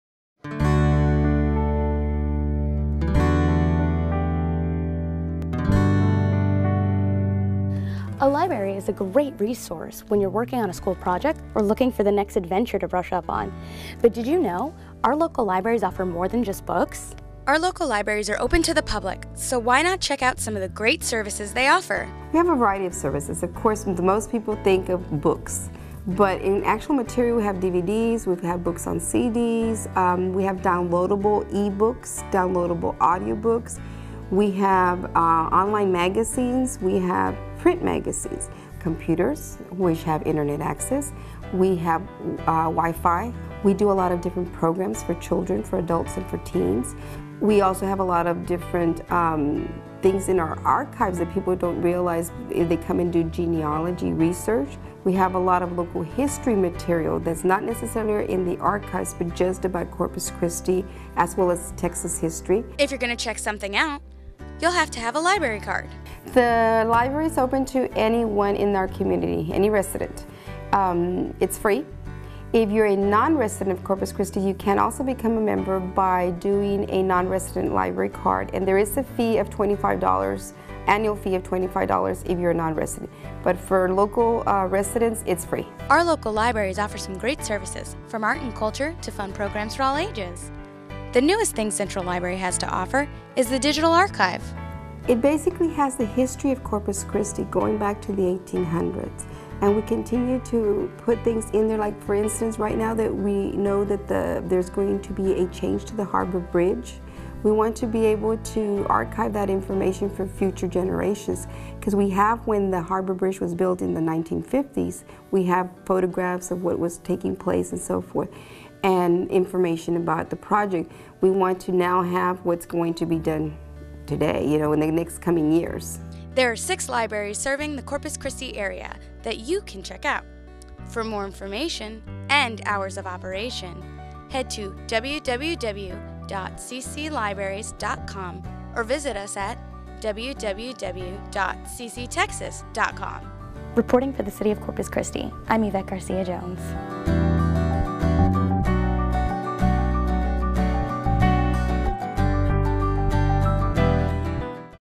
World at Your Fingertips at Corpus Christi Public Libraries
Need information for a school project? Looking for the next adventure? Or do you want to learn more about Corpus Christi's history? Along with books, magazines and DVDs, the Corpus Christi Public Libraries offer a variety of services including children's activities, local archives and internet access. Closed captioning by CCTV 20.